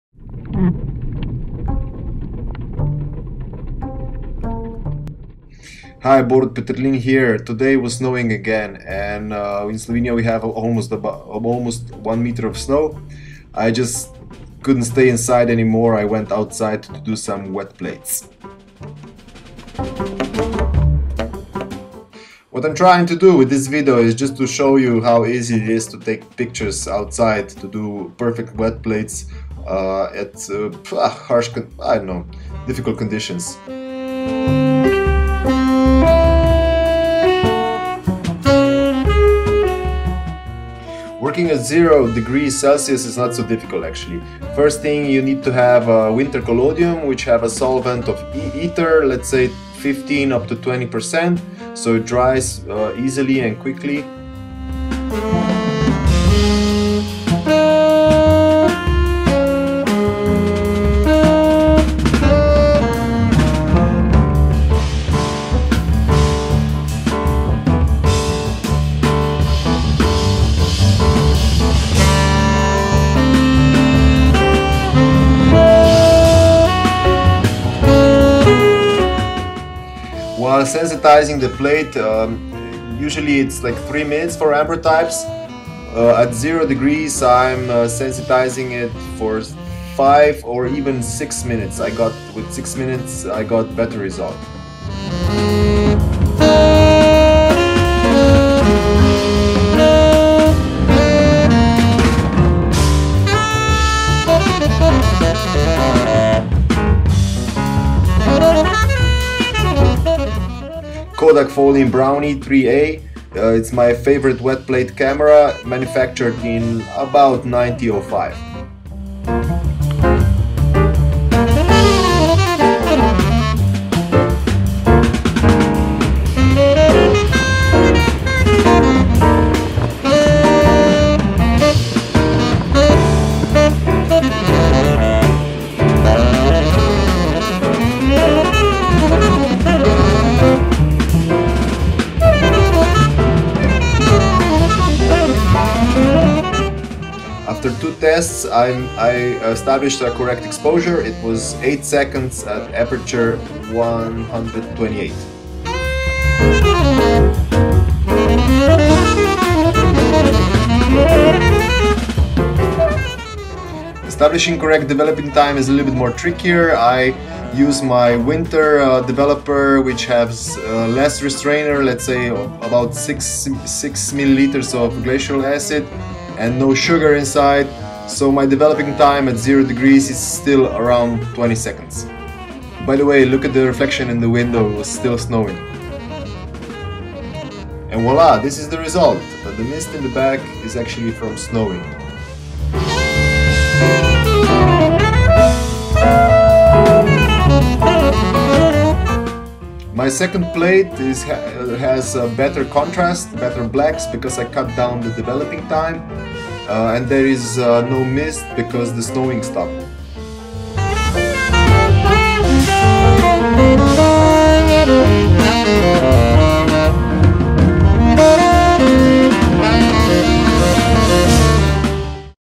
Wet Plate Collodion at temperature 0C
A somewhat tutorial video on doing wet plate collodion during winter at temperature 0C.